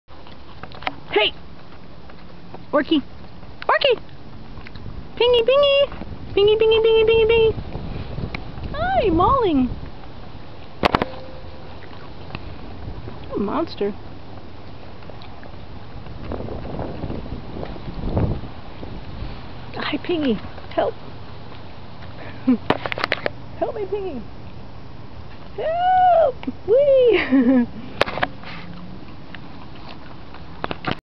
Orkie has issues
short video of Orkie being an Orca or Killer whale  she loves to bite